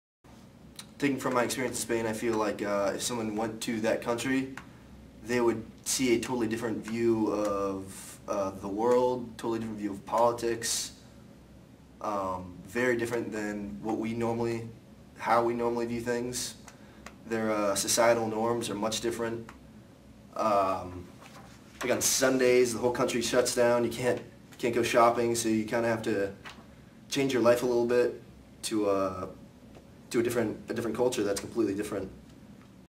What was it like living and communicating in a different culture? - Bobby (Spain)
Bobby talks about his experience studying abroad through University of North Florida's Coggin College of Business at Universidad Rey Juan Carlos in Madrid, Spain.